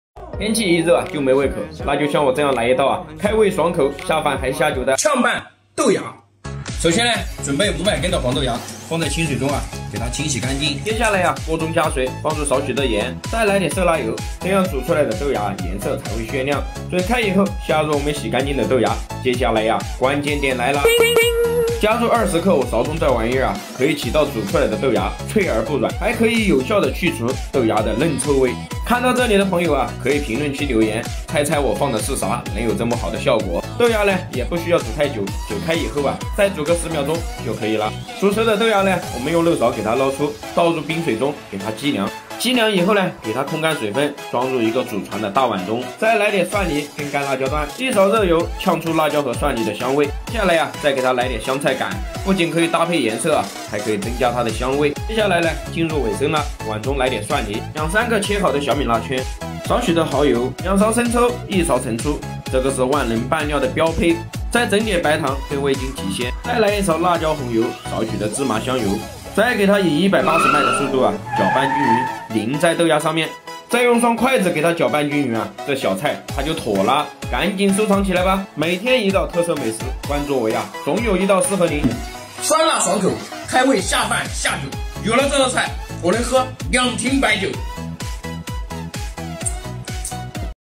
Soybean Sprouts in Sauce｜李厨教你「凉拌黄豆芽」的家常做法，步骤详细，爽脆开胃下酒又下饭！food
大家好，欢迎来到李厨记录生活YouTube官方频道！
分享美味家常菜！简单易学的做菜教程！
感谢大家的喜爱！